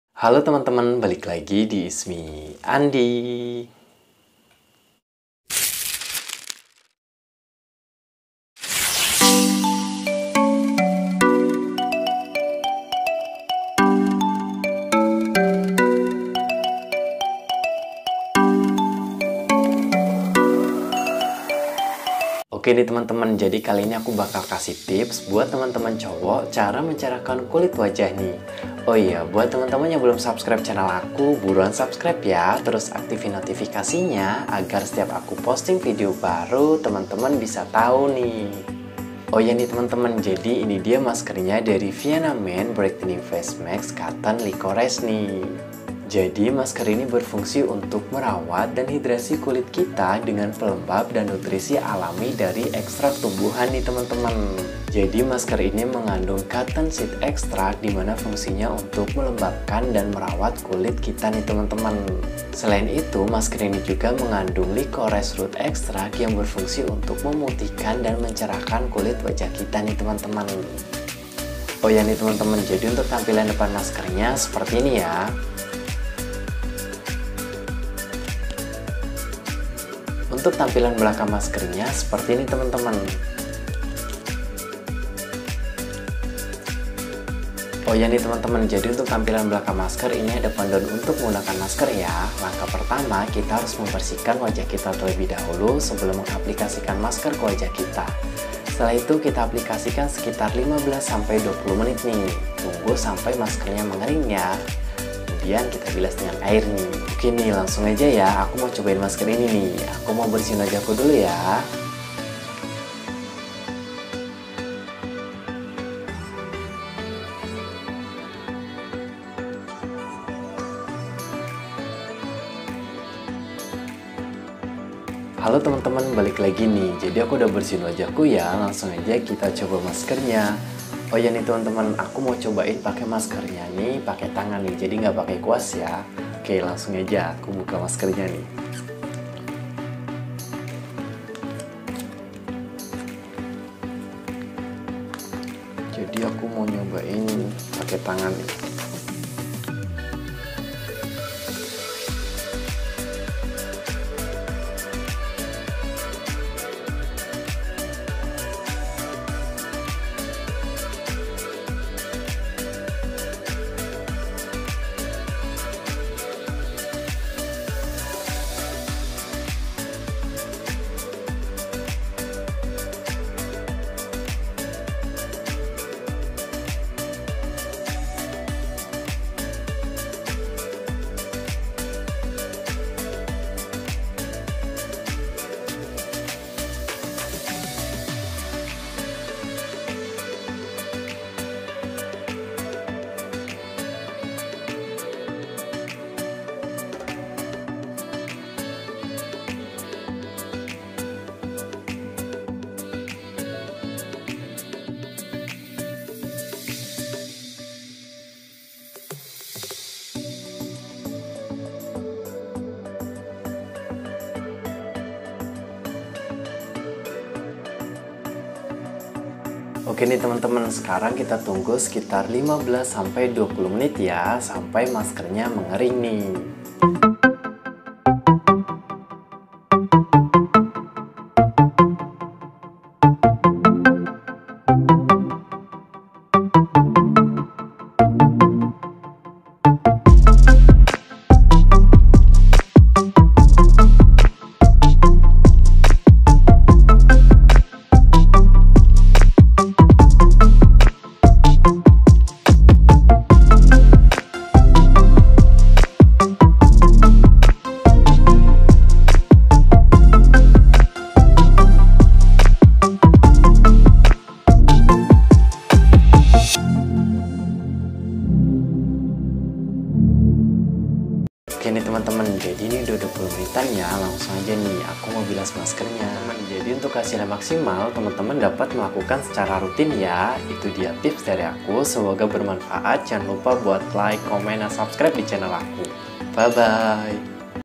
cara ampuh - mencerahkan wajah pria (masker wajah untuk pria) Vienna Men Whitening Mask
Halo temen temen kali ini Andy bakalan kasih tau masker pencerah wajah untuk cowok nih❤️ yaitu Vienna Men Whitening Mask Cotton Licorice❤️

Merawat, menghidrasi kulit dengan pelembab dan nutrisi dari ekstrak tumbuhan. Cotton seed extract untuk melembabkan dan merawat kondisi kulit. Licorice root extract membantu agar kullit tampak putih dan cerah. Kulit senantiasa lebih halus, lembut dan berseri-seri.

🔴Gunakan 1-2x dalam seminggu dengan olesan tipis pada wajah dan leher. Relaks dan diamkan 15-20 menit hingga kering.  Bilas hingga bersih

SEMOGA BERMANFAAT YA TEMEN TEMEN 😊